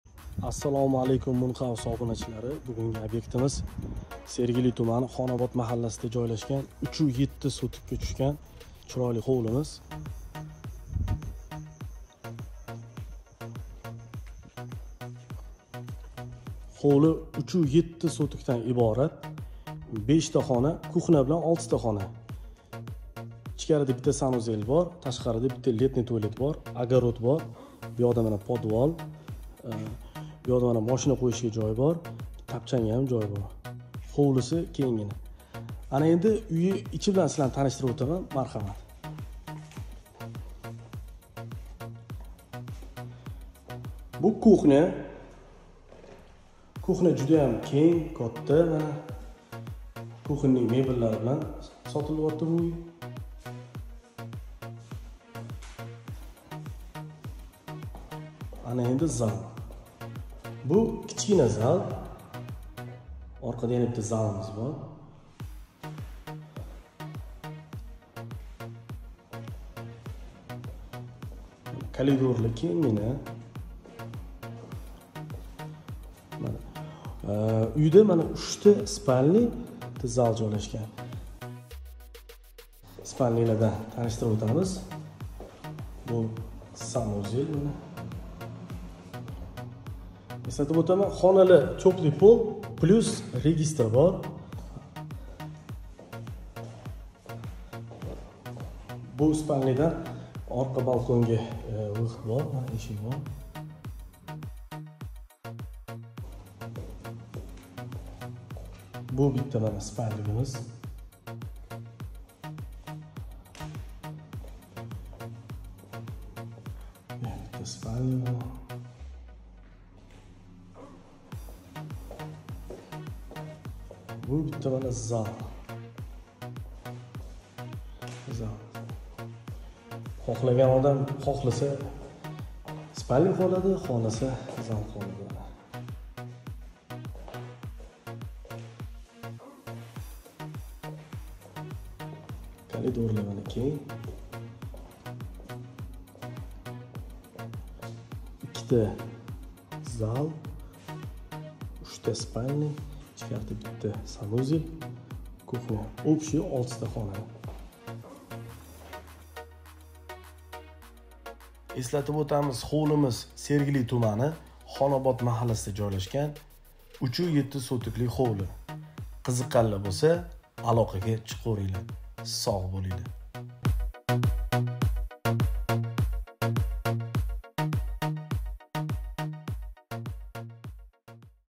⛔️СОТИЛДИ!⛔️ 115.000$ га 3.7 сотокли ховли сотилади!
🏠«Под Ключ» тайёр ховли сотилади!
✅ Умумий майдони: 3.7 соток
✅ Хоналар сони: 5 хона
✅ Санузел: 2 та
✅ Ховлимизда катта кухня, коридор, балкон, подвал, огород, гараж бор!
📍Район: Сергили тумани
📍Ориентир: Хонобод махалласи
💲Нархи: 115.000$